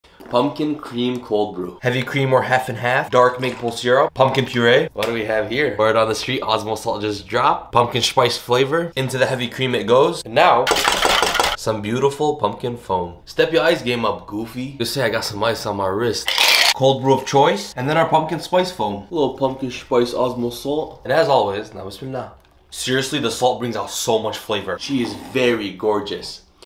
Pumpkin Cream Cold Brew | The Golden Balance
Pumpkin Cream Cold Brew 🥵

1/2 cup heavy cream
1/4 cup dark maple syrup (preference)
1/4 cup pumpkin purée
1-2 tsp @osmosalt pumpkin spice
1 tsp Vanilla extract optional
Cold brew of choice